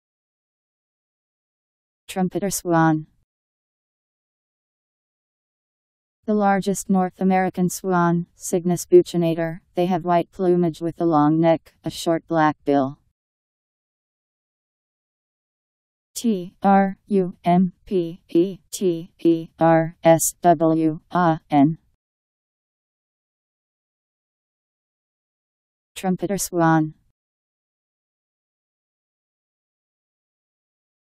Trumpeter swan Meaning
Video shows what trumpeter swan means. The largest North American swan, Cygnus buccinator; they have white plumage with a long neck, a short black bill..  trumpeter swan pronunciation. How to pronounce, definition by Wiktionary dictionary. trumpeter swan meaning. Powered by MaryTTS